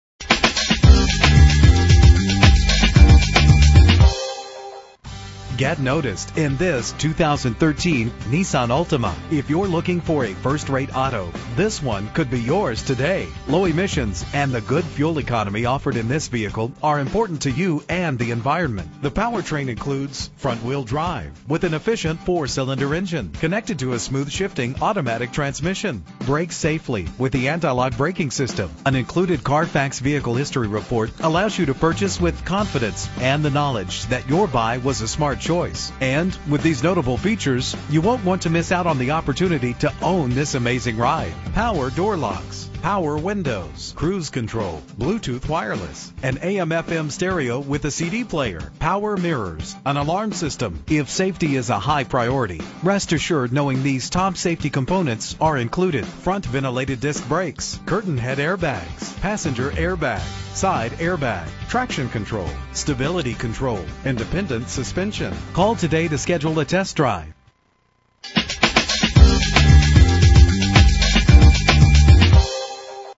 2013 Nissan Altima - Little Falls NJ
Year: 2013
Make: Nissan
Model: Altima
Trim: 2.5 S
Engine: 2.5 liter inline 4 cylinder 16 valve
Transmission: Automatic CVT
Color: Super Black
Mileage: 28681
Address: